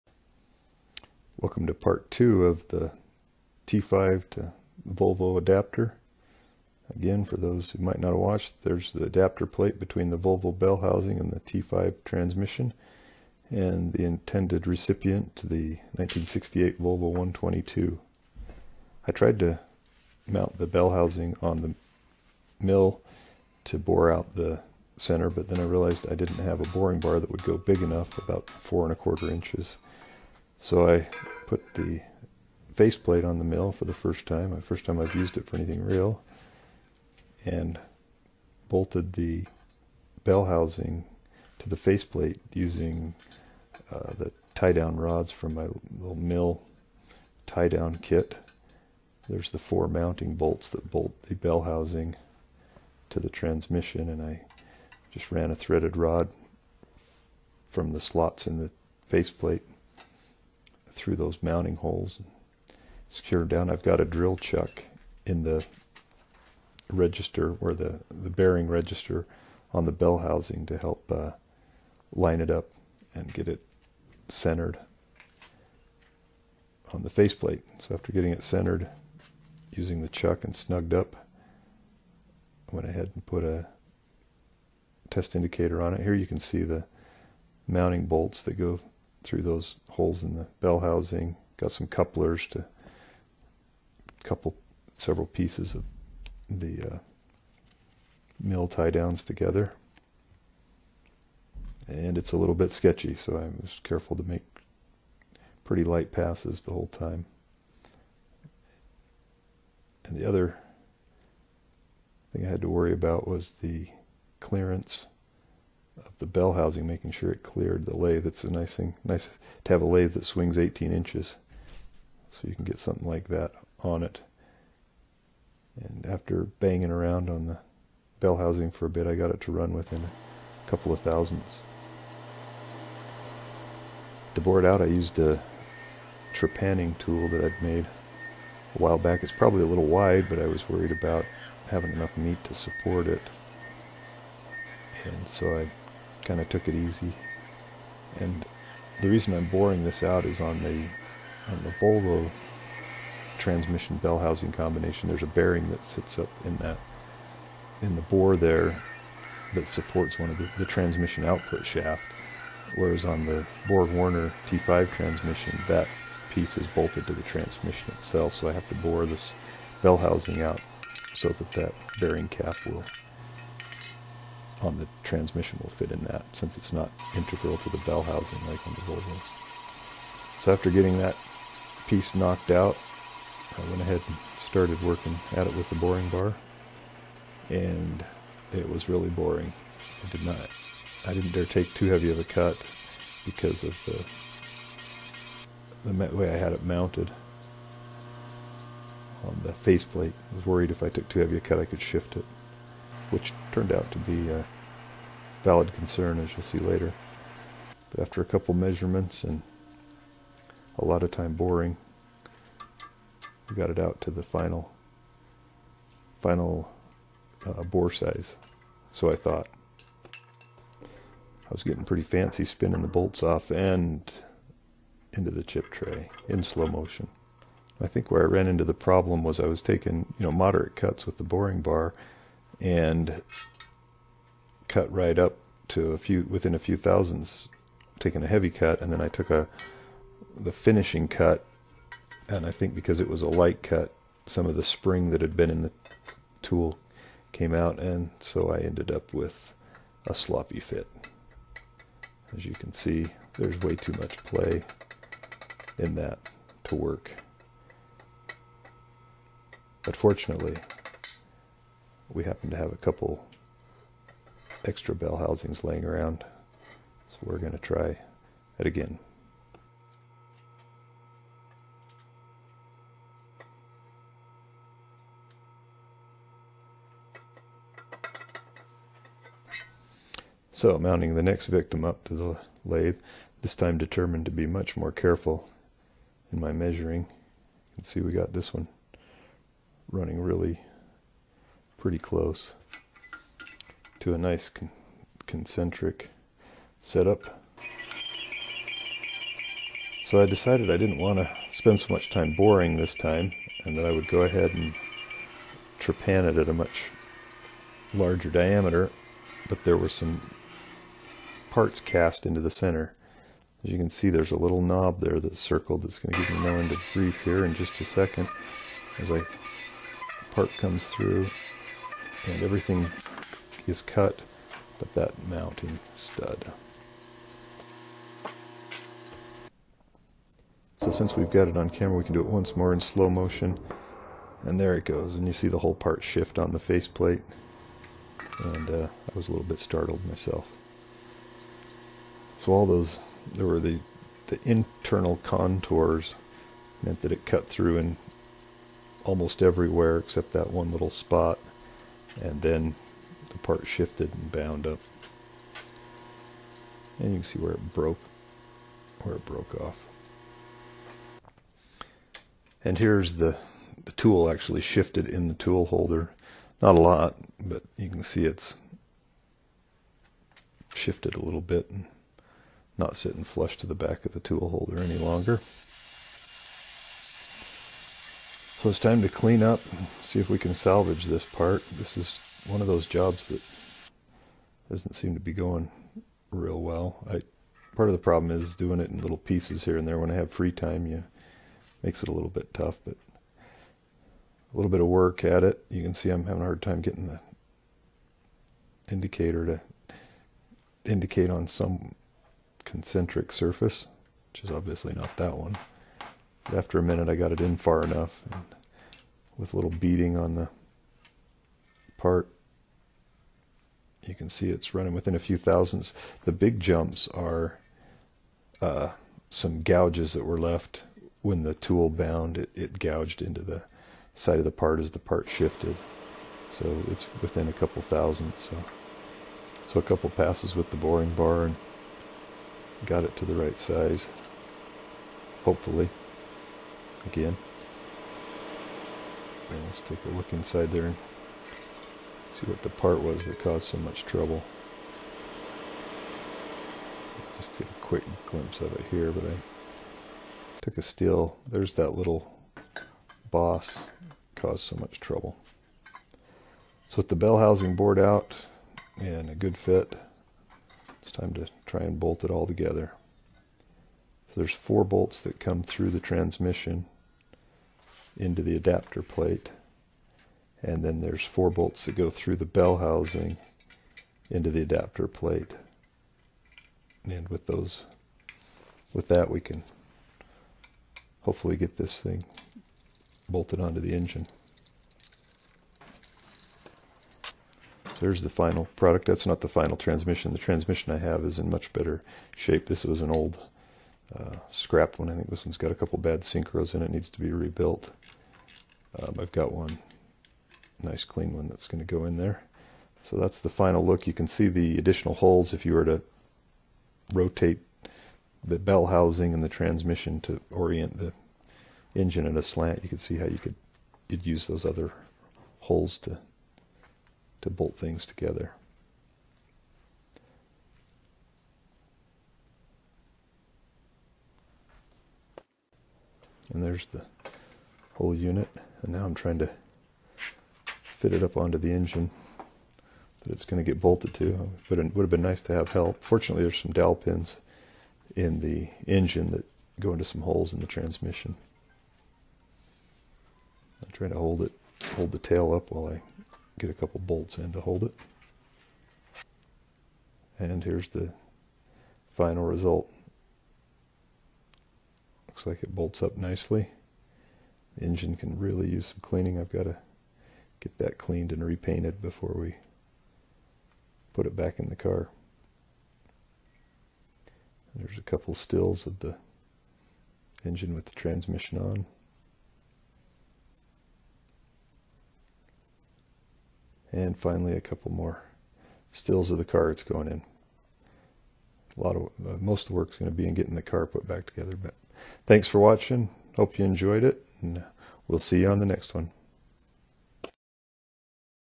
T5-Volvo Adapter: A Hendey LAthe Project, Part 2
As a bit of an upgrade I decided to install a B230FT out of a 1989 740 Turbo and as I am hoping to increase the stock horsepower a bit a stronger transmission was in order. The common swap is a Borg Warner (now Tremec) T5 from a Ford Mustang. In order to mate the two I needed a transmission adapter plate and to bore the bellhousing to fit the T5. If you need an adapter you can buy them much cheaper than making your own.

As usual, below is a list of YouTube machinists that I follow and recommend anyone who is interested in machining or the creative process go take a look.